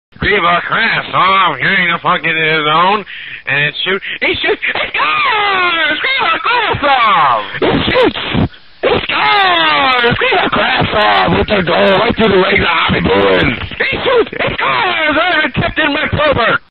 Foley-Bramowicz
Eric doing his best Pat Foley circa 1997